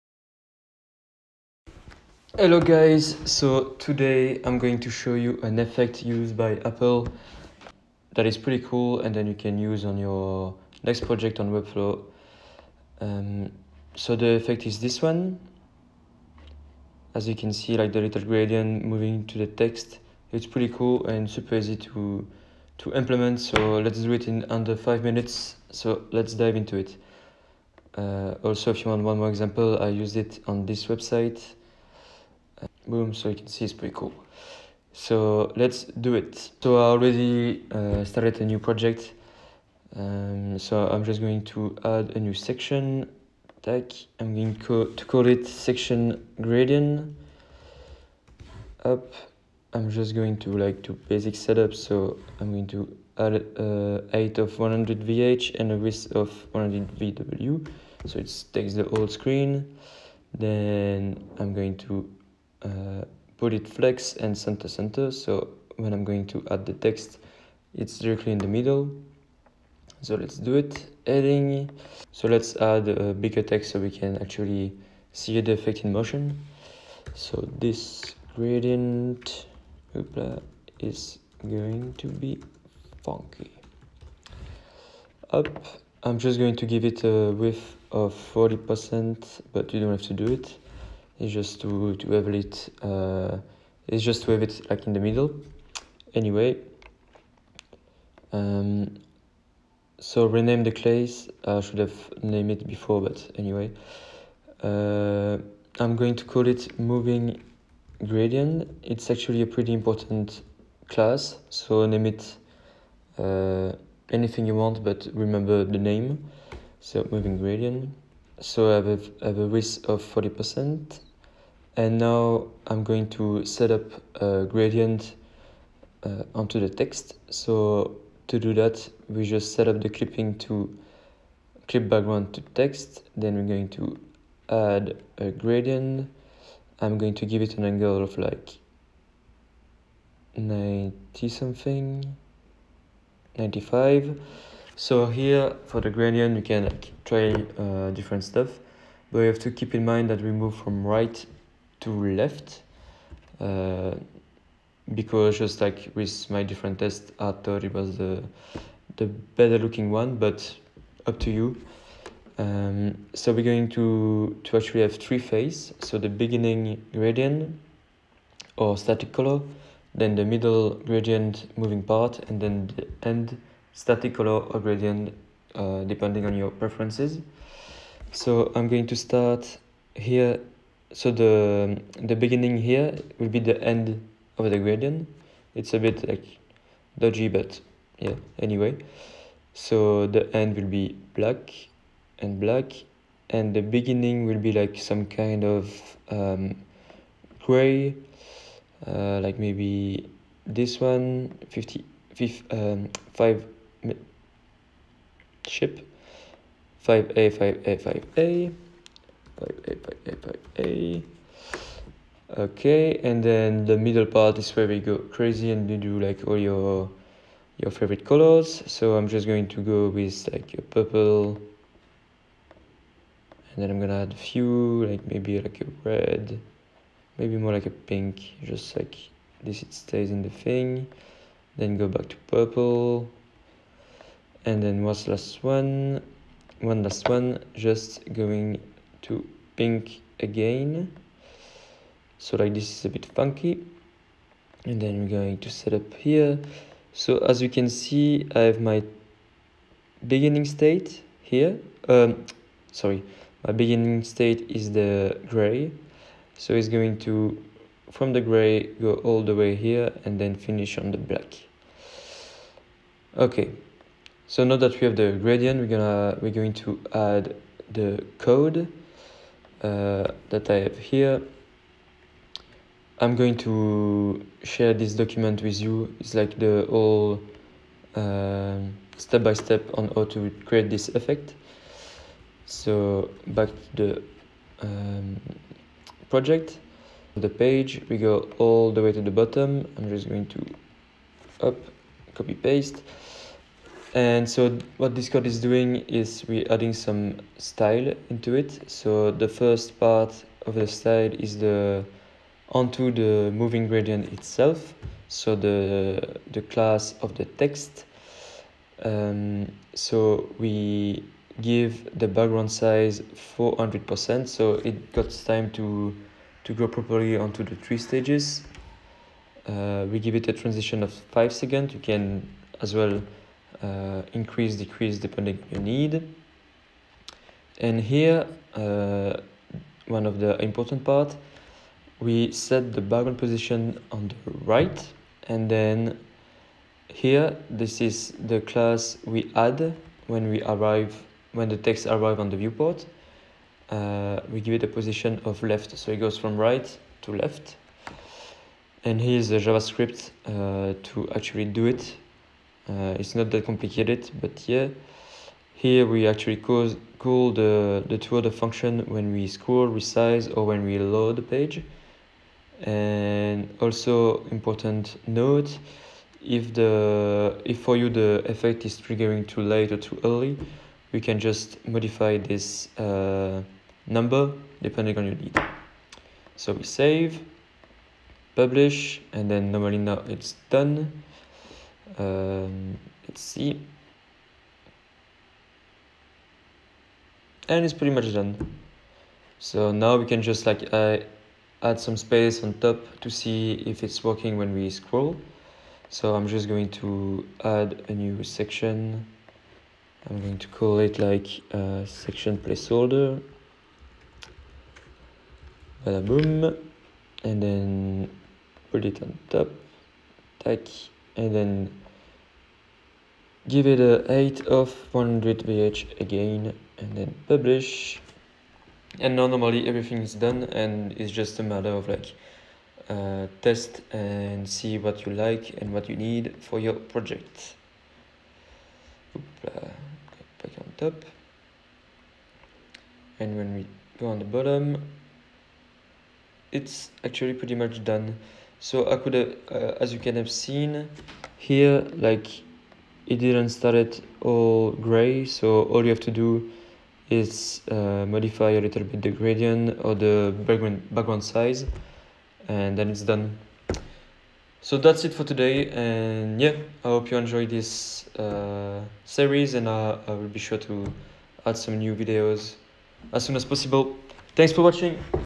Apple Moving Text Gradient | Webflow Quick Snippet
You are starting with Webflow?

Hey guys, here is a cool effect for your next webflow project.
It is super easy to use and it can be done in less than 5 minutes.

This is my first video, I hope you enjoy it!